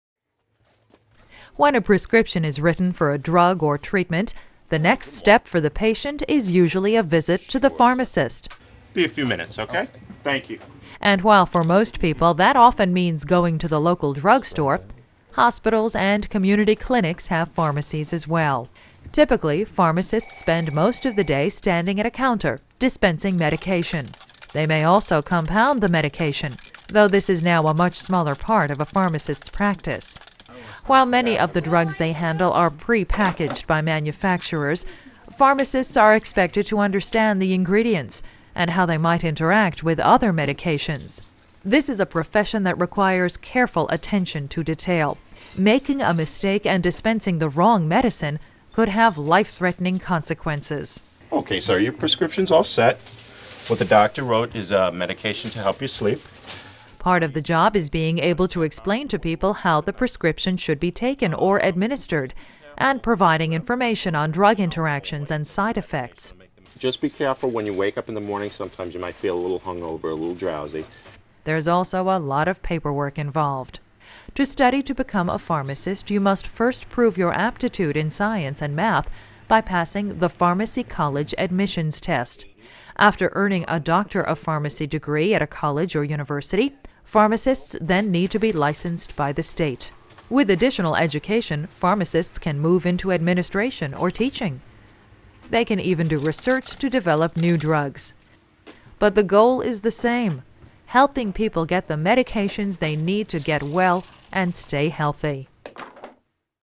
Pharmacists - Career Profile
, Anonymously evaluating every job, every day for you.

Compound and dispense medications following prescriptions issued by physicians, dentists, or other authorized medical practitioners.